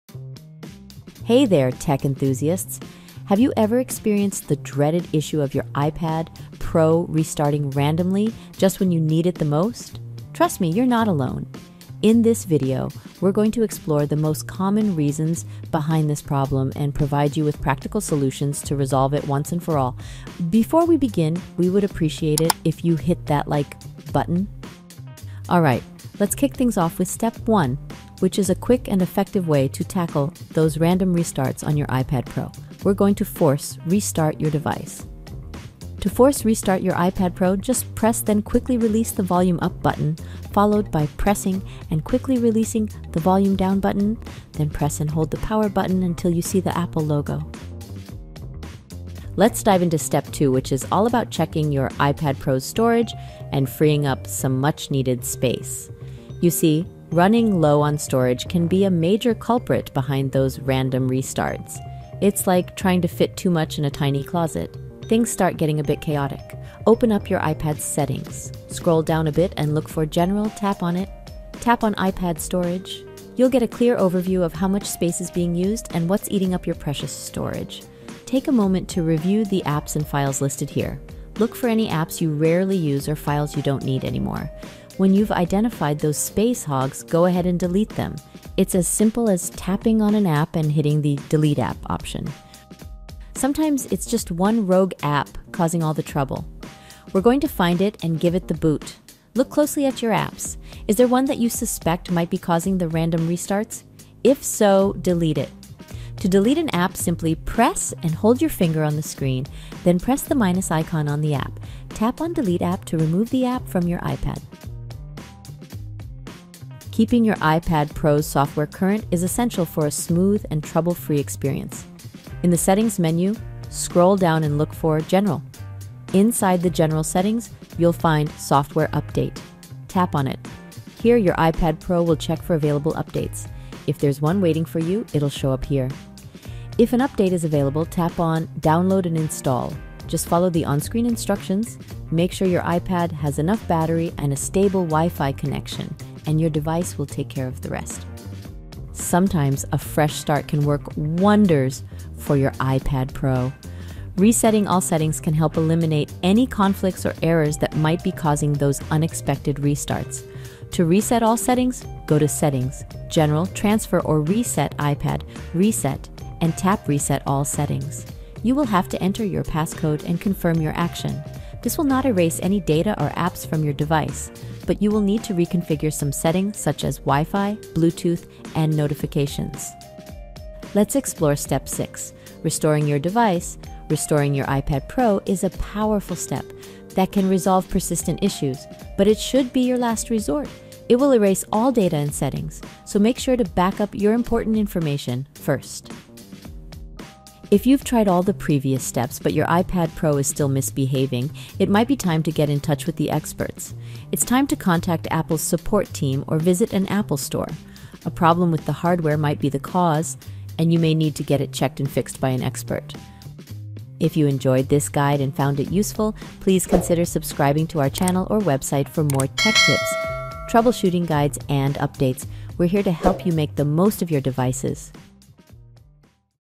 How to Fix iPad Pro Restarting Randomly
Are you frustrated with your iPad Pro randomly restarting? You're not alone! In this comprehensive guide, we'll explain why this happens and provide you with troubleshooting steps to fix it. Keep your iPad Pro running smoothly with these tips!